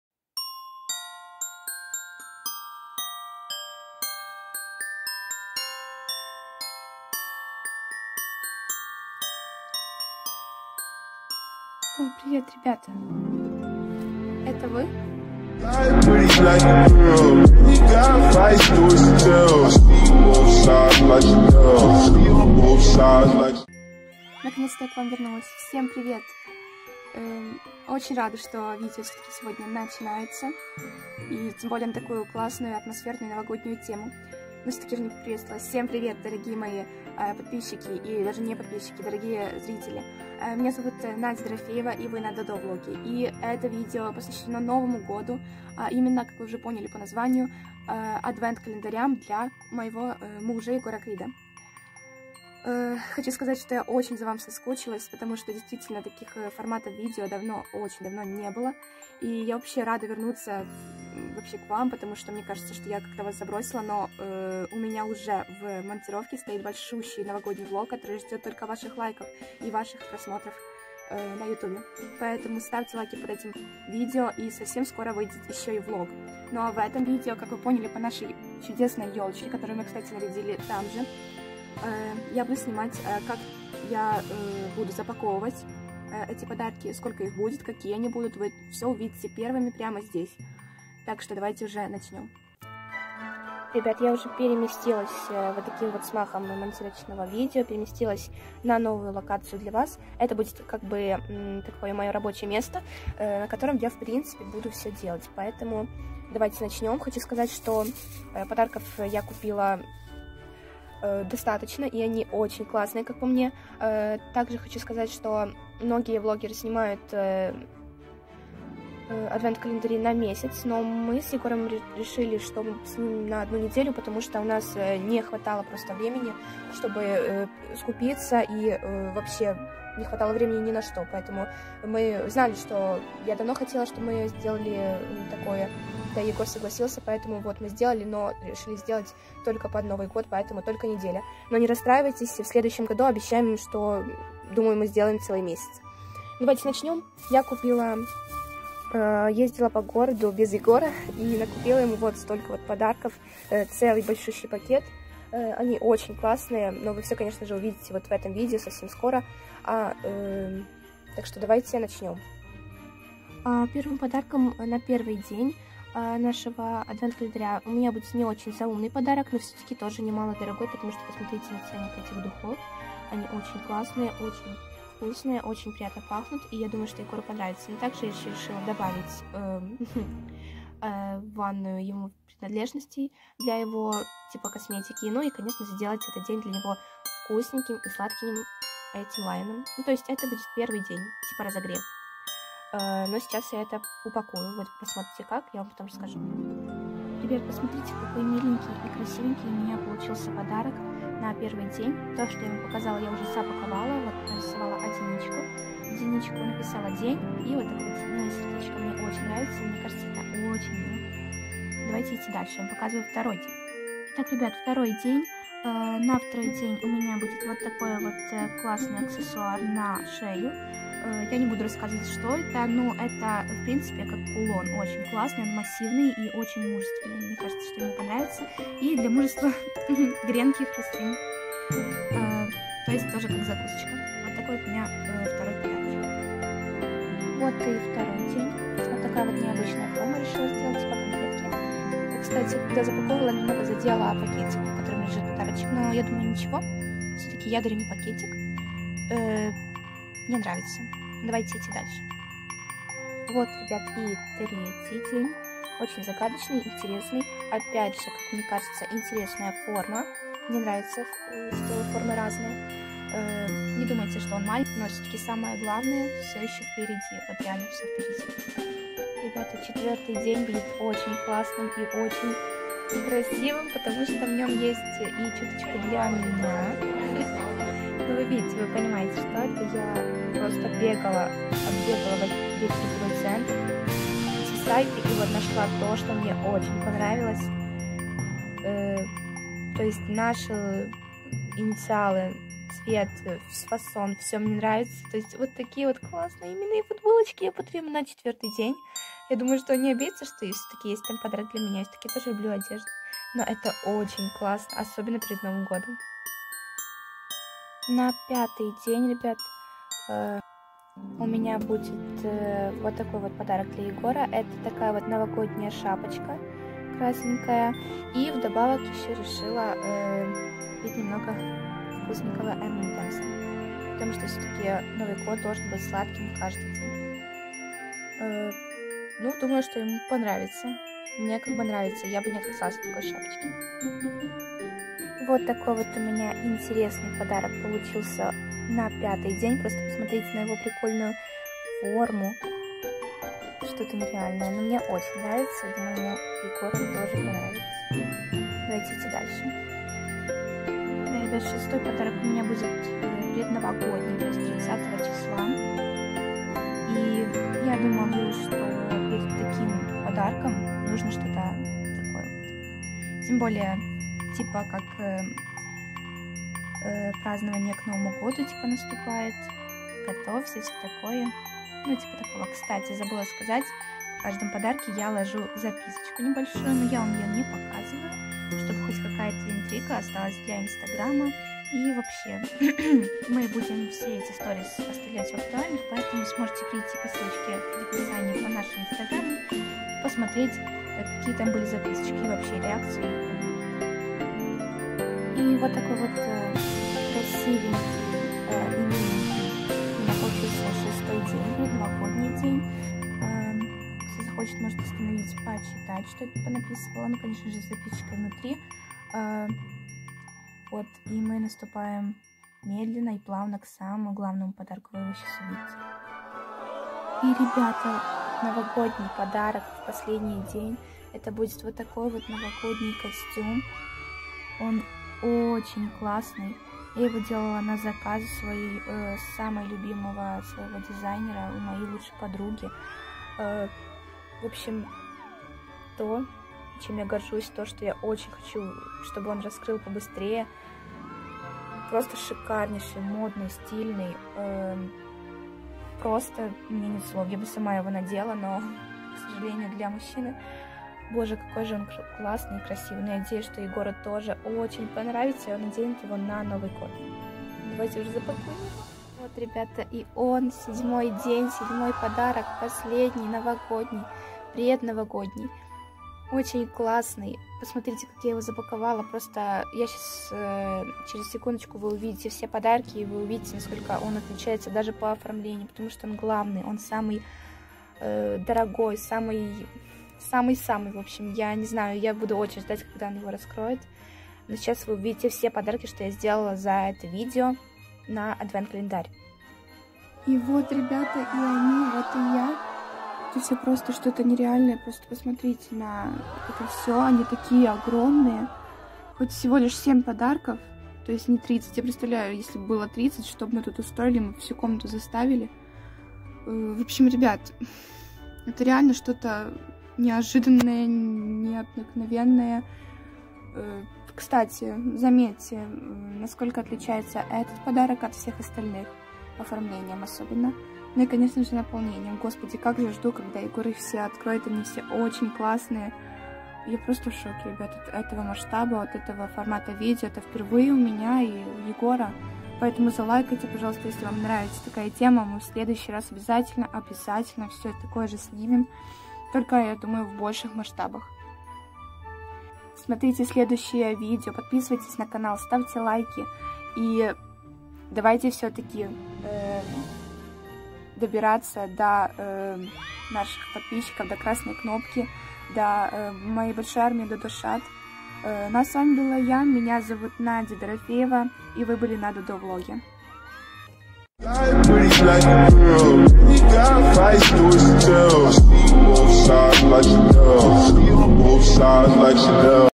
Адвент календарь 2020. Запаковка подарков. Что же будет в последнем? DODOVLOG
Всем привет! В етом видео я буду упаковывать подарки для новогоднего адвент календаря,  который предназначен для моего мужа. Етот адвент будет 7-ми дневным,  поетому будет 7 подарков. Не уверен ? Поммотри ето видео и сам убедишся в етом. Ваша DODO.